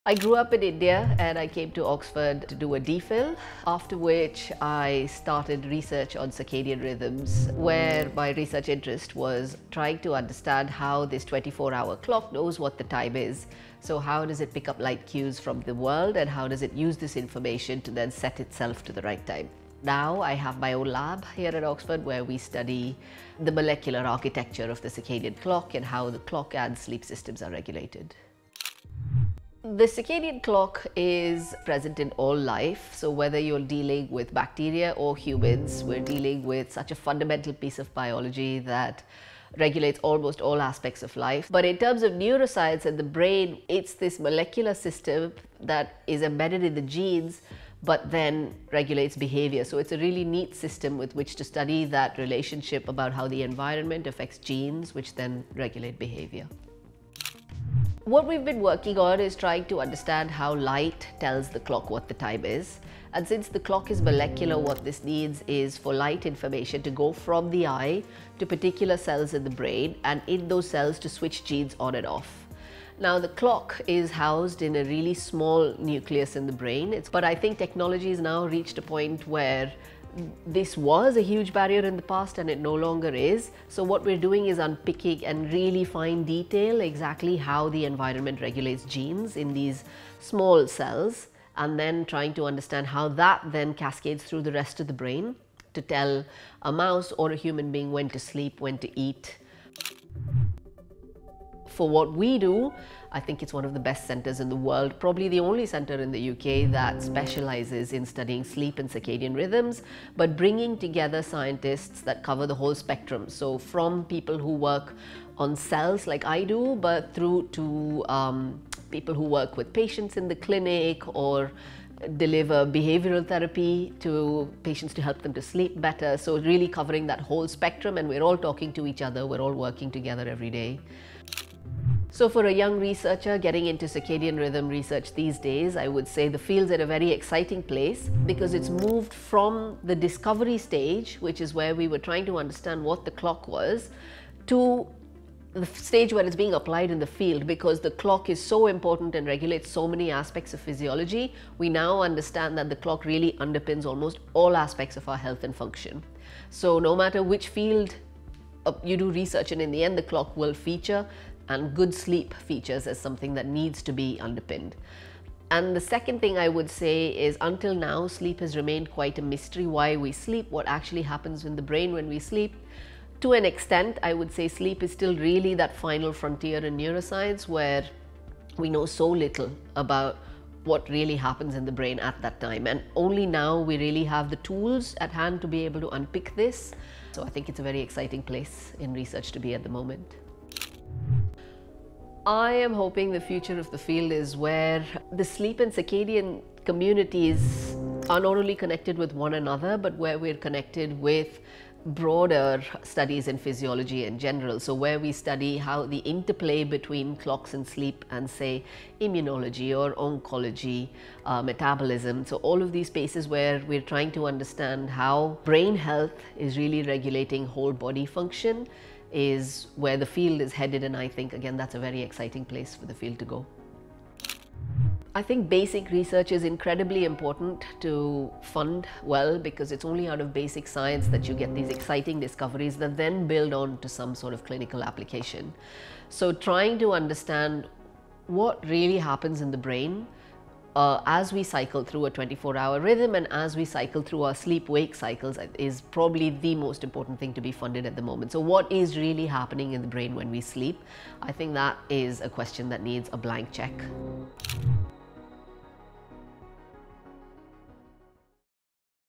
Studying the biology of the circadian clock: Dr Aarti Jagannath
Associate Professor Aarti Jagannath, from Oxford's Sleep and Circadian Neuroscience Institute, discusses her career and her research into the biology of the circadian clock.

Find out more about Oxford's work on brain and mental health